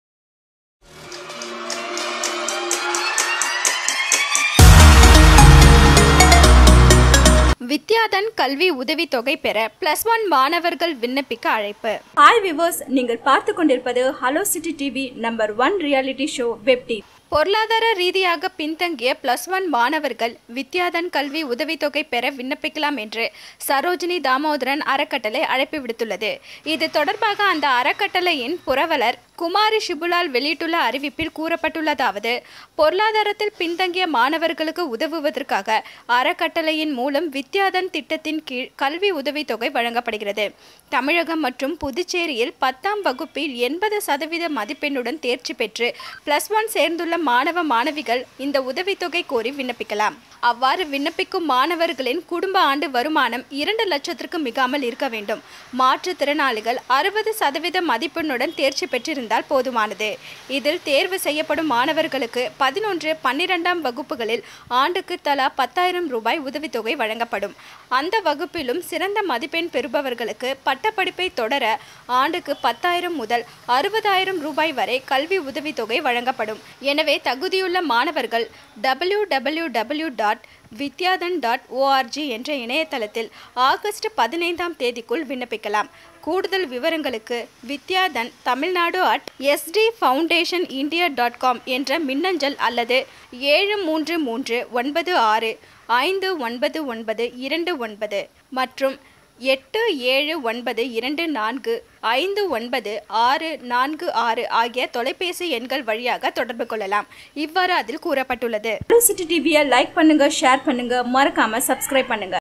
வித்யாதன் கல்வி உதவித்தொகைபெற பிளஸ்-1மாணவர்கள் விண்ணப்பிக்க அழைப்பு | Vidyadhan Scholarship | School
வித்யாதன் கல்வி உதவித்தொகை பெற பிளஸ்-1 மாணவர்கள் விண்ணப்பிக்க அழைப்புVidyadhanScholarship